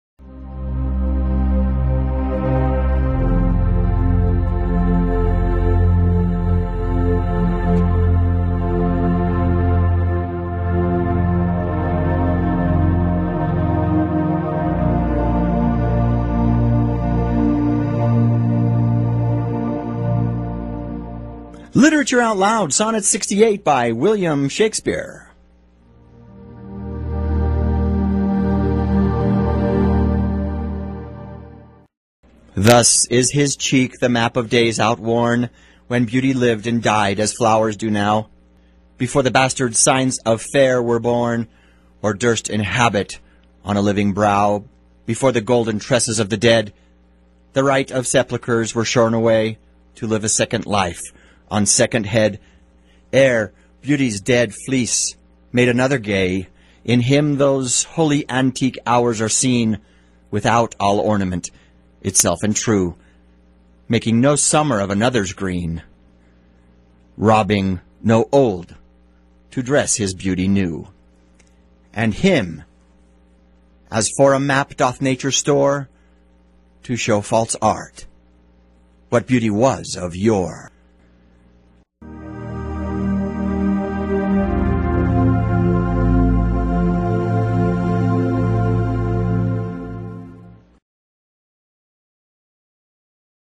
Sonnet 68 by William Shakespeare
Sonnet LXVIII

Thus is his cheek the map of days outworn,

When beauty lived and died as flowers do now,

Before the bastard signs of fair were born,

Or durst inhabit on a living brow;

Before the golden tresses of the dead,

The right of sepulchres, were shorn away,

To live a second life on second head;

Ere beauty's dead fleece made another gay:

In him those holy antique hours are seen,

Without all ornament, itself and true,

Making no summer of another's green,

Robbing no old to dress his beauty new;

And him as for a map doth Nature store,

To show false Art what beauty was of yore.

LITERATURE OUT LOUD -- see and hear great literature

Audio narrations with synchronized visual text